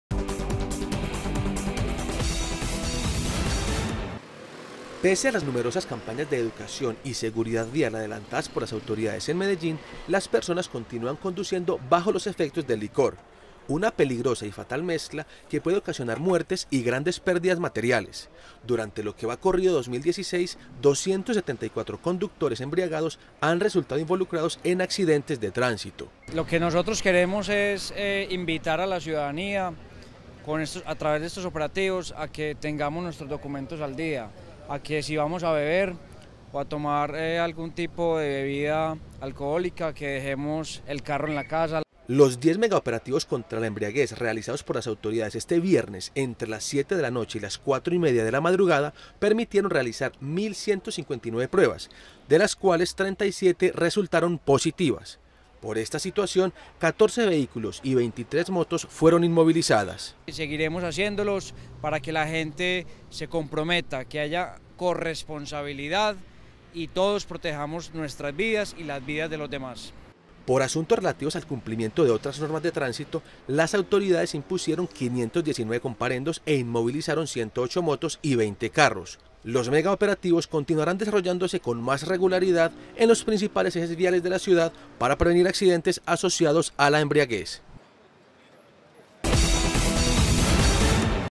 Se realizaron operativos contra la embriaguez en Medellín - Teleantioquia
Hasta las 4:30 a.m. del viernes se extendieron los operativos contra la embriaguez en 10 puestos de control instalados en diversas vías estratégicas de Medellín. Durante lo que va corrido del año las autoridades han desarrollado 106 megaoperativos en la ciudad, en los cuales han examinado más de 26 mil conductores.